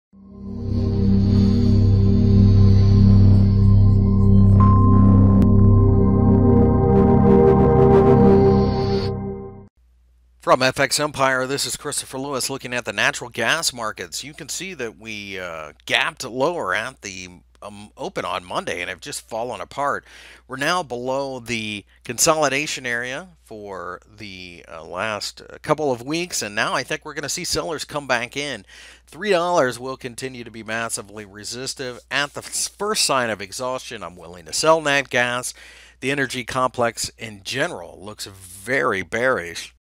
Natural Gas Technical Analysis for June 20 2017 by FXEmpire.com
Natural gas markets gap lower at the open on Monday, breaking below the three dollars level significantly. For the full article go - Natural Gas Forecast June 20, 2017, Technical Analysis.Find more information about Forex and Commodities News Technical Analysis Fundamental Analysis and Brokers on FX Empire